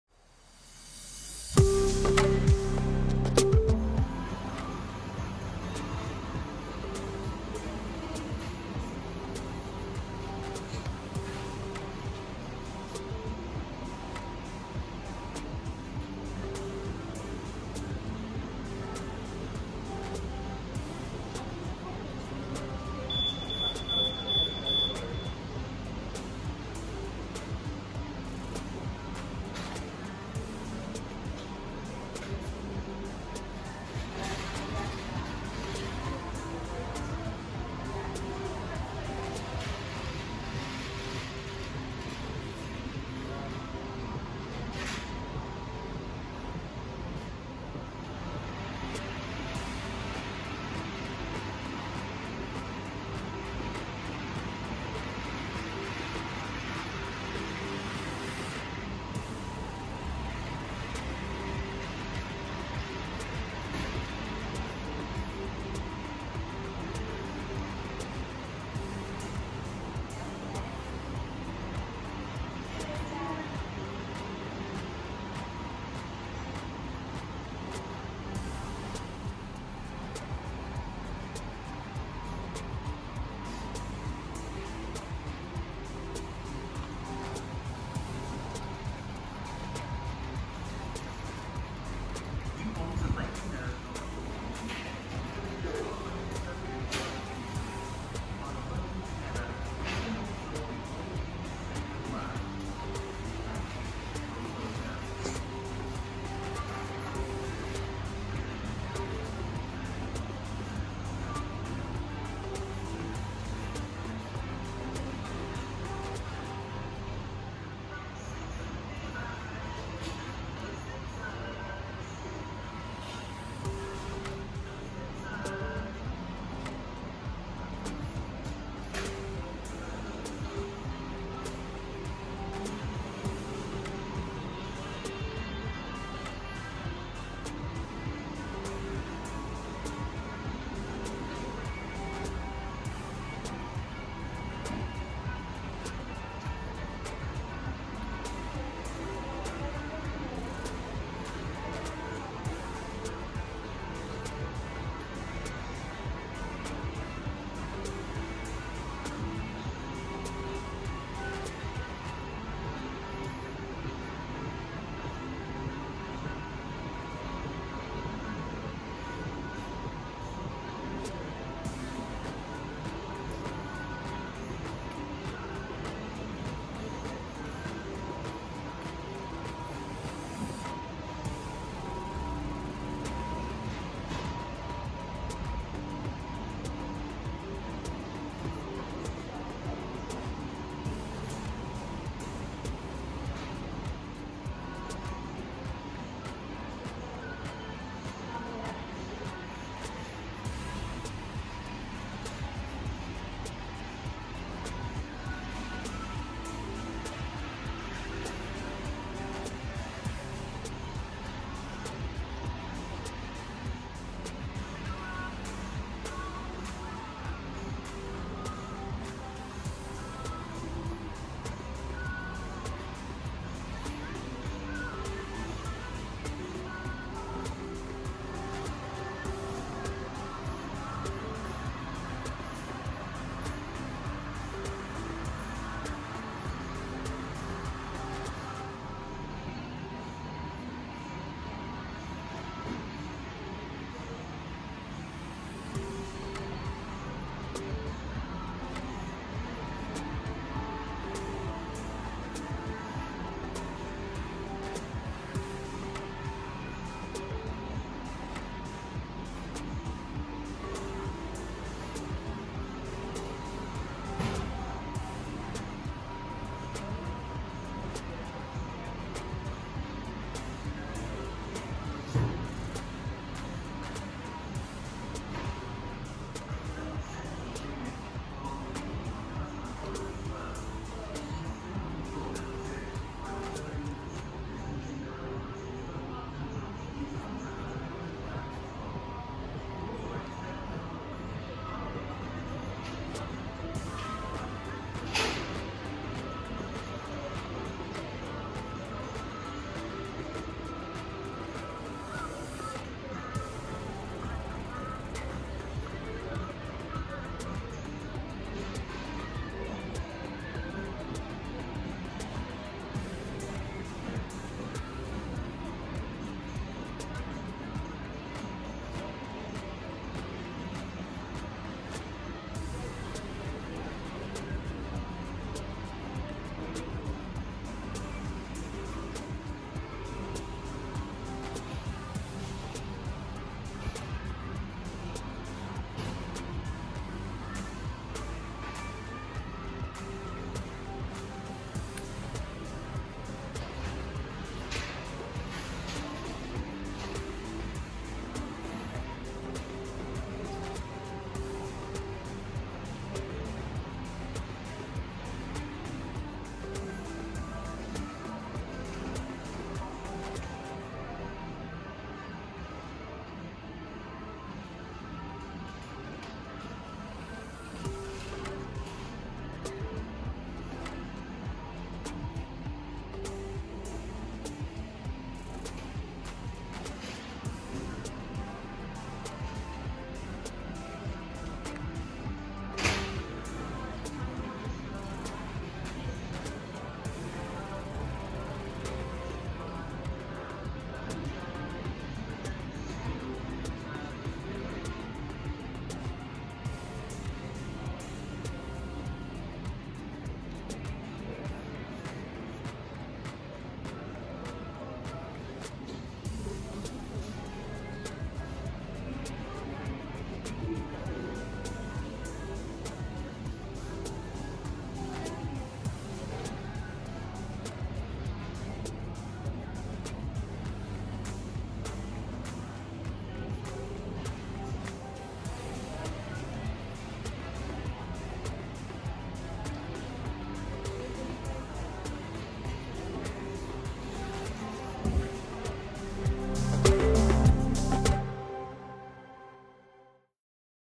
NEW - Homeware, Cleaning Products, Garden, Kitchenware - What’s available in Wilkinsons - May 2020
Join us for a look around what’s available in Wilko’s currently, we went in for some cleaning products and lightbulbs and they were out of both things we needed! However still a good range of availability in gardening, homeware and some paint!